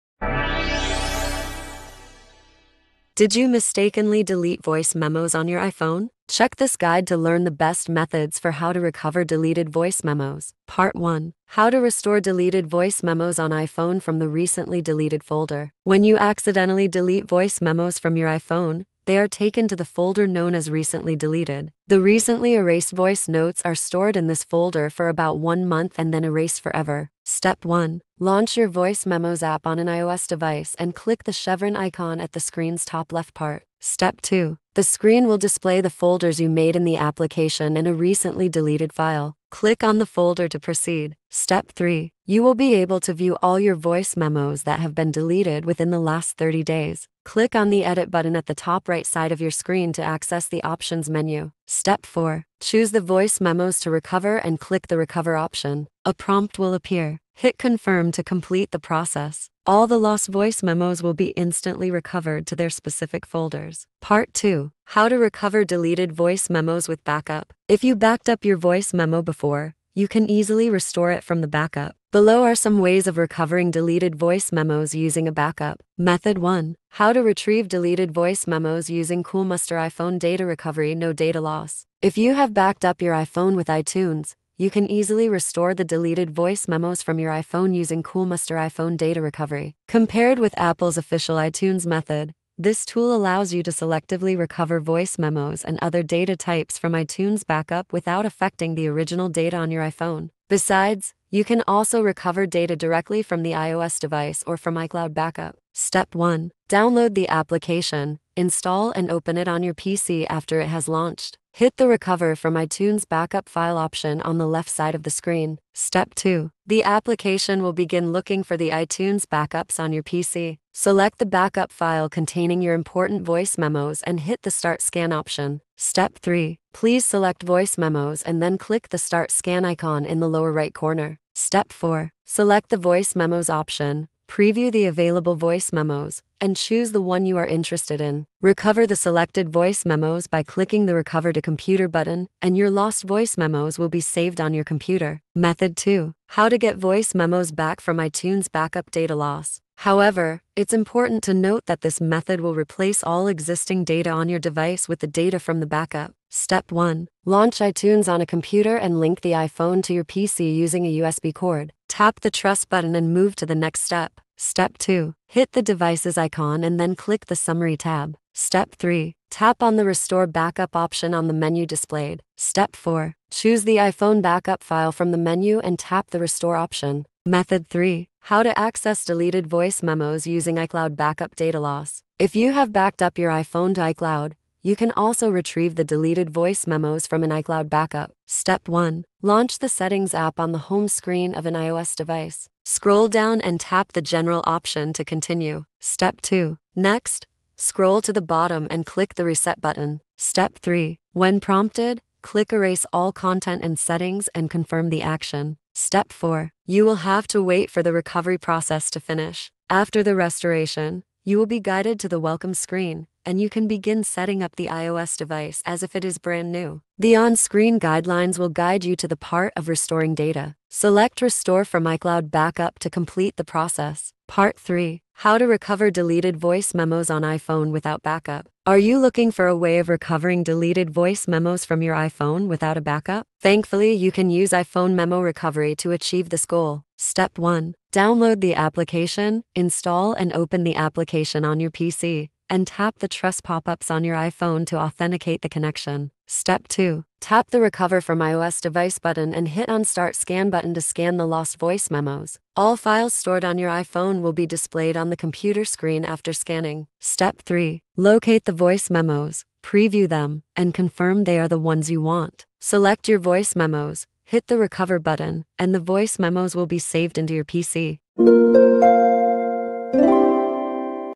How to Recover Deleted Voice Memos on iPhone? [5 Easy Ways]
Did you mistakenly delete voice memos on your iPhone? Check this guide to learn the best methods for how to recover deleted voice memos.

00:00 Start
00:10 Part 1. How to Restore Deleted Voice Memos on iPhone from the Recently Deleted Folder
01:03 Part 2. How to Recover Deleted Voice Memos with Backup
01:15 Method 1. How to Retrieve Deleted Voice Memos Using Coolmuster iPhone Data Recovery (No Data Loss)
02:25 Method 2. How to Get Voice Memos Back from iTunes Backup (Data Loss)
02:57 Method 3. How to Access Deleted Voice Memos Using iCloud Backup (Data Loss)
03:44 Part 3. How to Recover Deleted Voice Memos on iPhone Without Backup